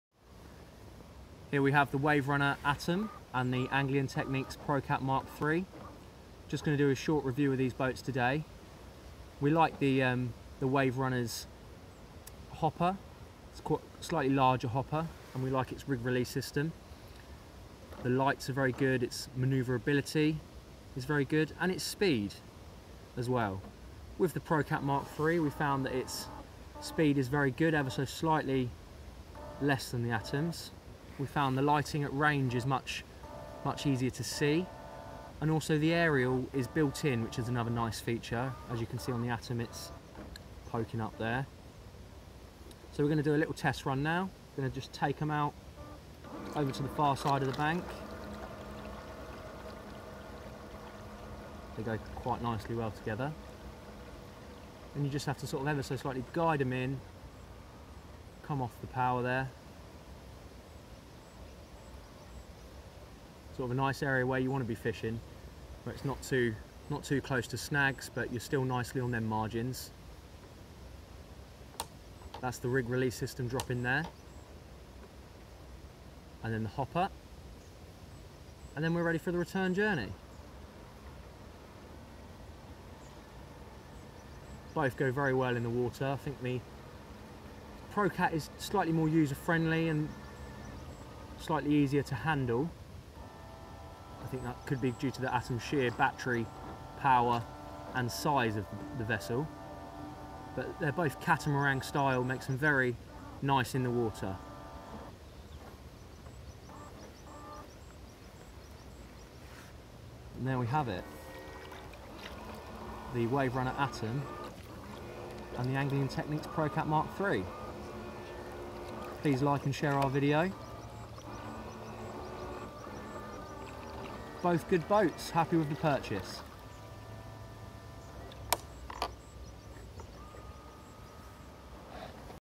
Bait Boats Waverunner Atom and Angling Technics Procat MK3
A short review of the bait boats designed for fishing.